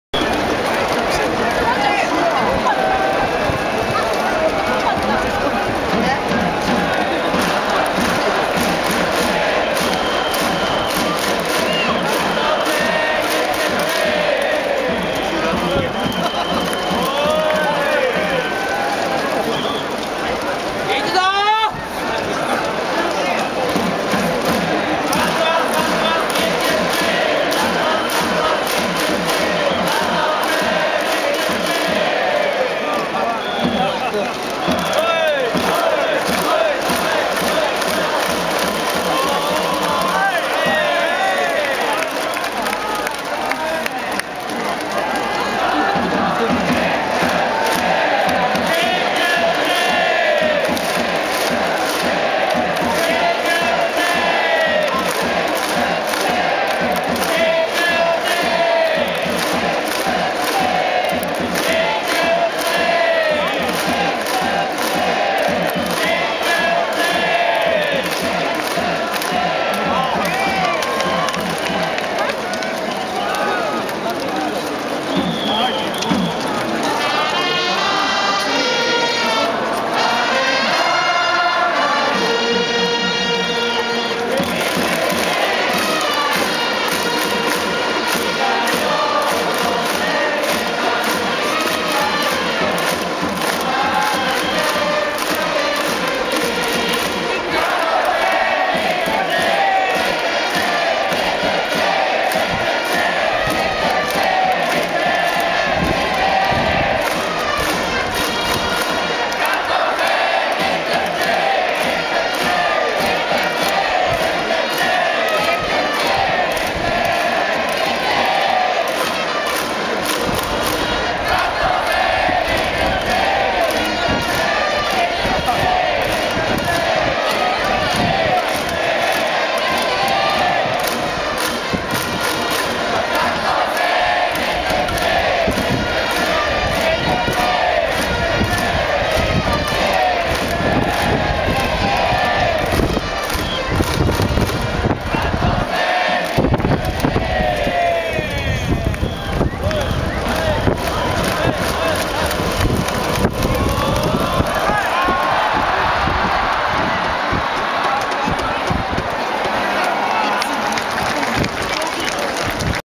【2018 公式戦】広島カープ 菊池涼介のバッティングフォーム_2018052001
2018 公式戦 マツダスタジアムでの対東京ヤクルトスワローズ戦、広島カープ 菊池涼介のバッティングフォームです。